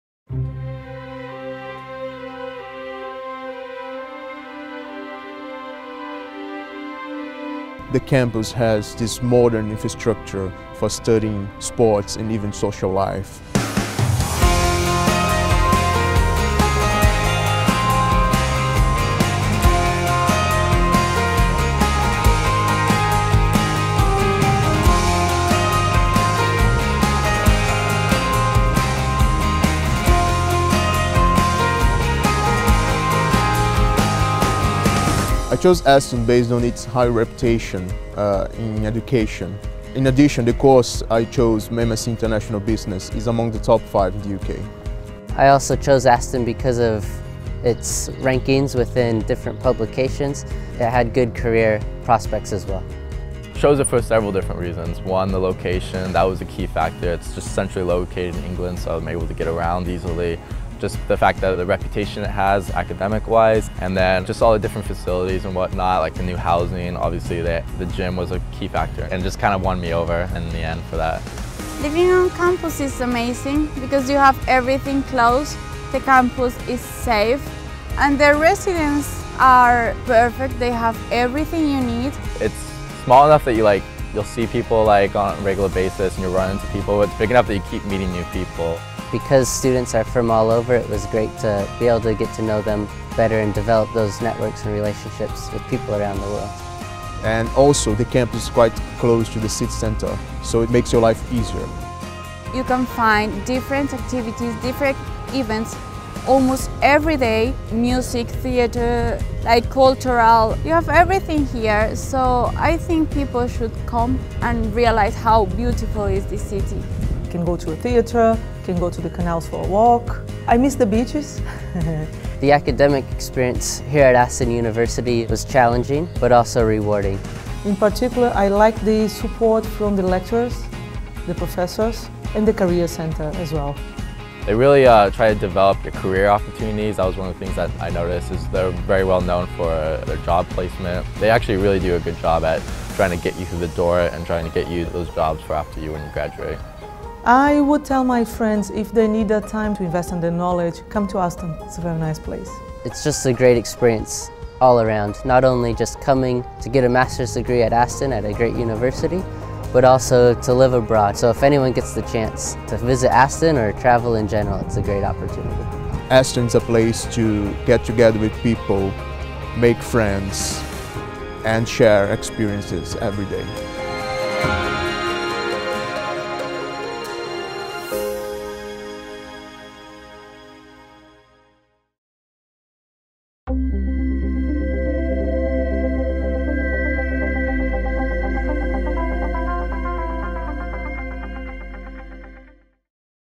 Aston University International Student Voices - The Americas
Aston University is a top 35 UK institution, with 90% student satisfaction, beating many Russell group institutions. We have an excellent international reputation, being a home to students from around the world.

We understand it may be a bit daunting to think of moving half way around the globe to study. So here are the words of someone who has dared to do so.  We hope to see you soon and welcome you to the Aston family.